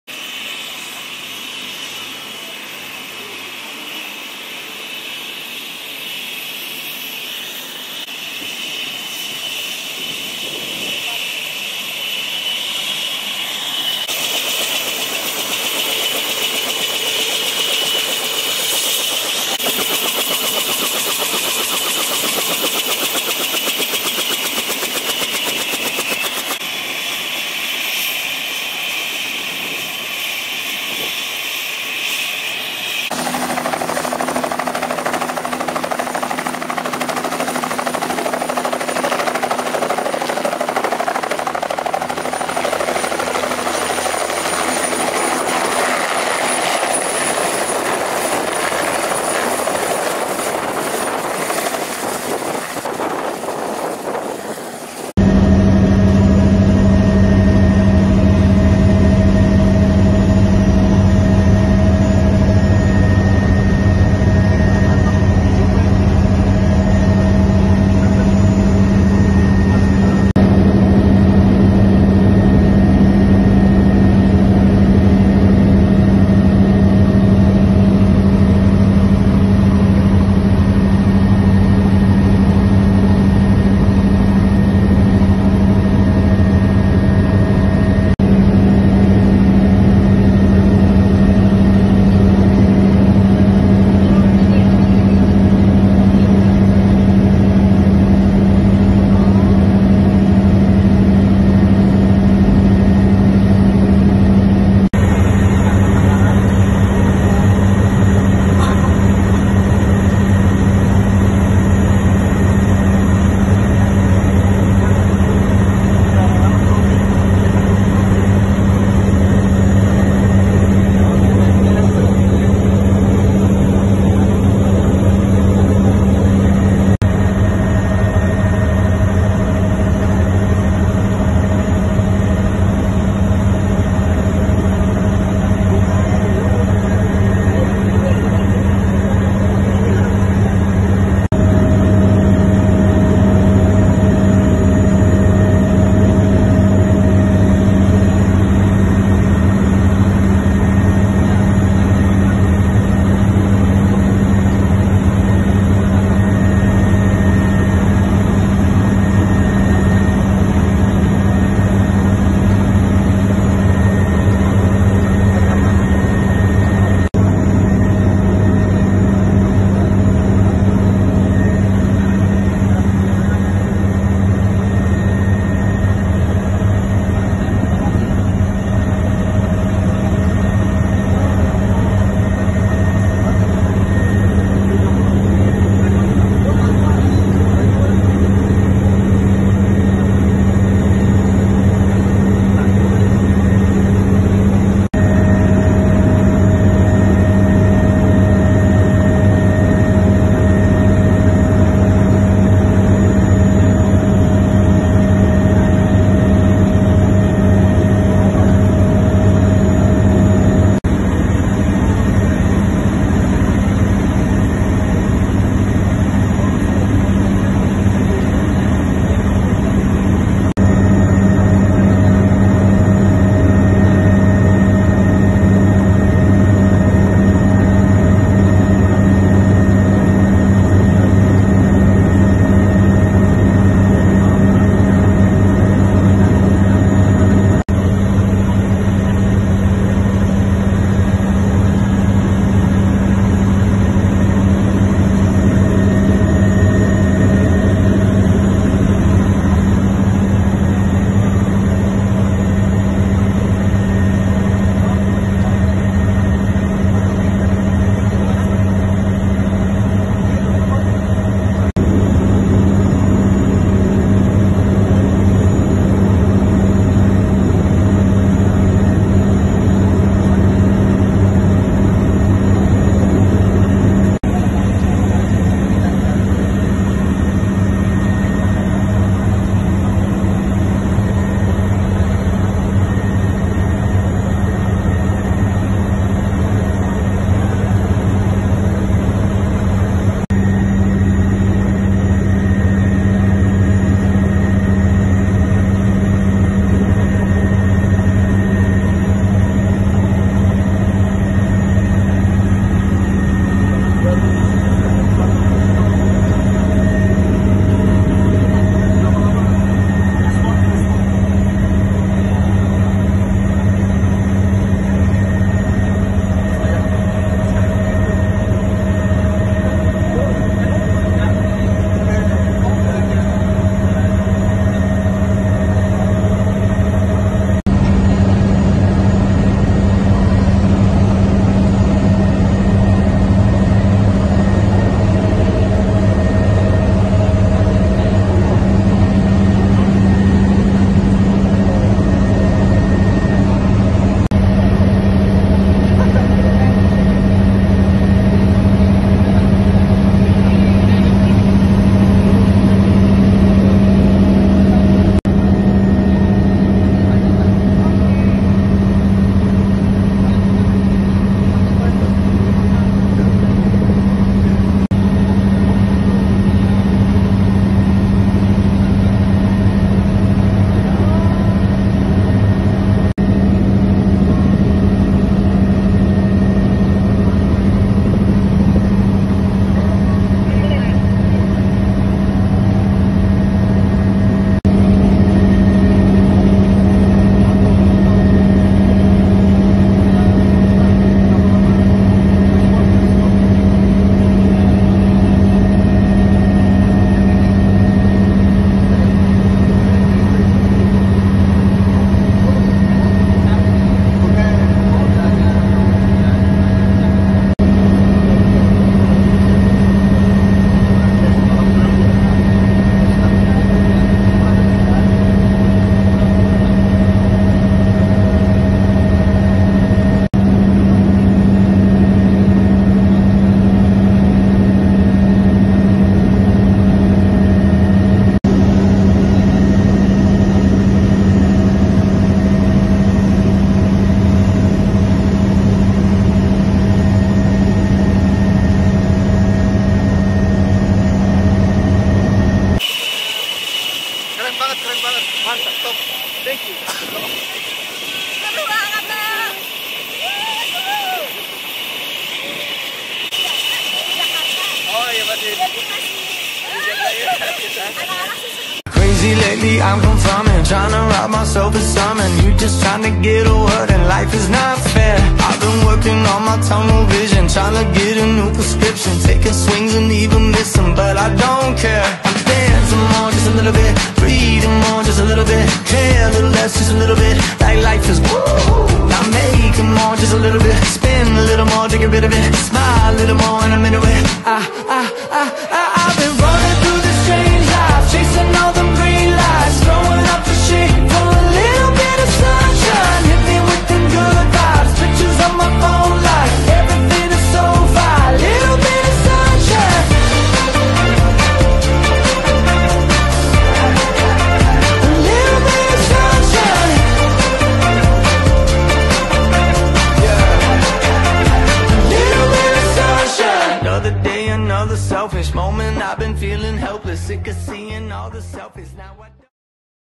KELILING BALI NAIK HELI
Terbang keliling pantai Bali naik Helikopter